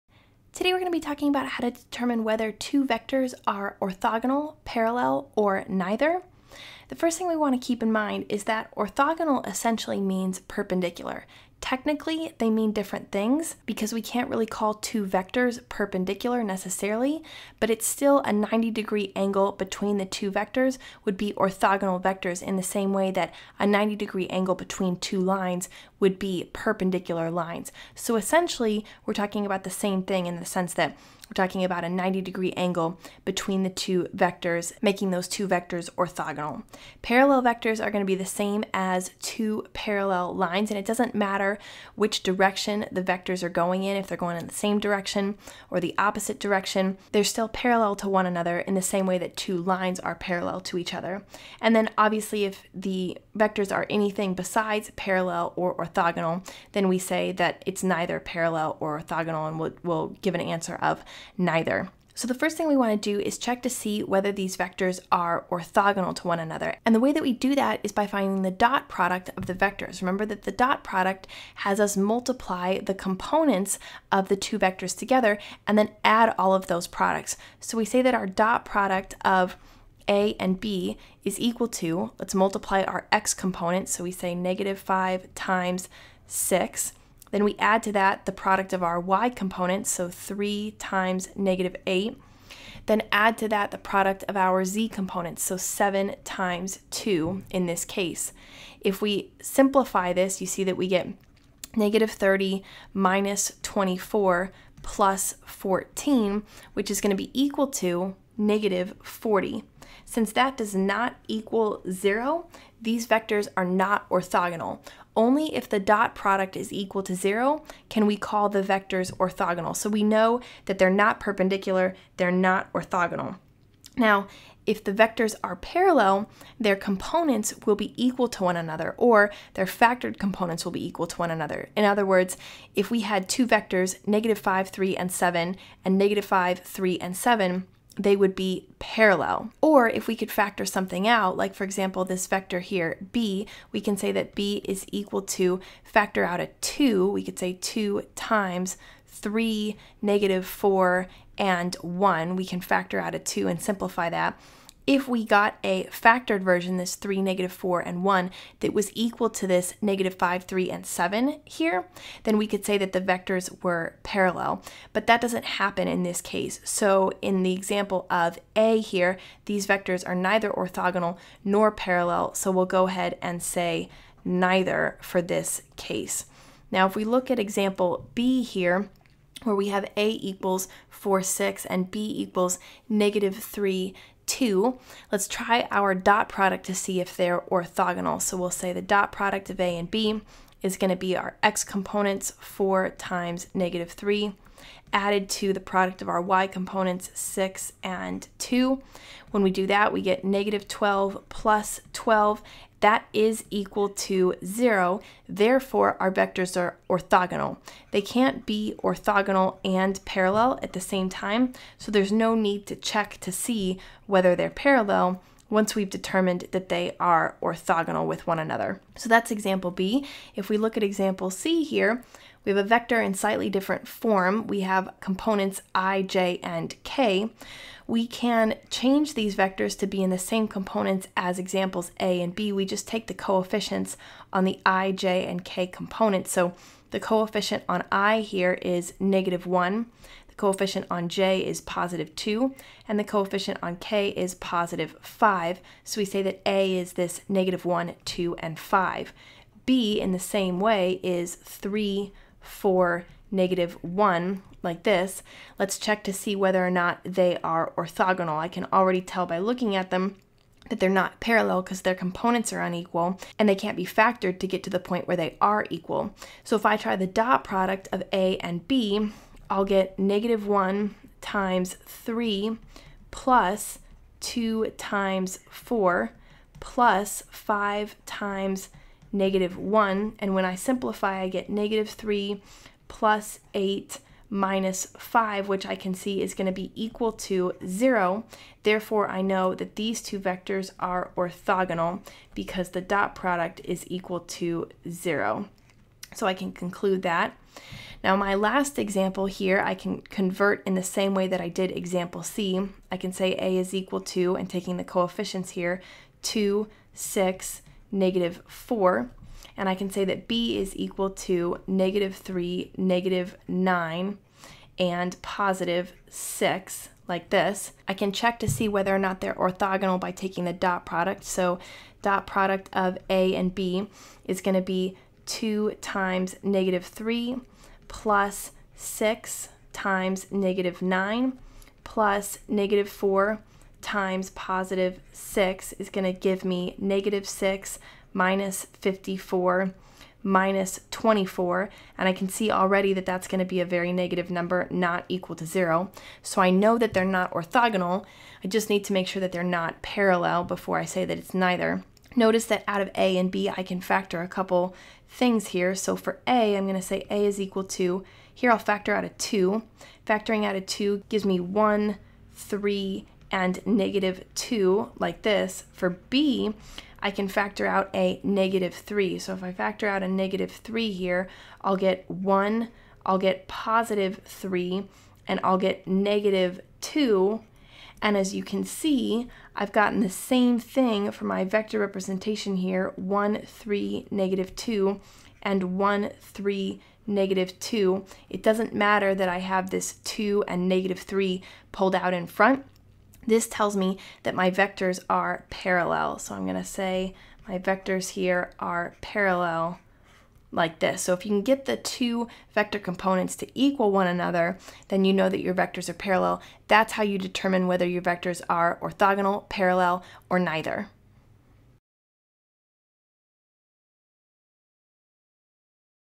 Orthogonal, parallel or neither (vectors) (KristaKingMath)
Learn how to determine whether two vectors are orthogonal to one another, parallel to one another, or neither orthogonal nor parallel.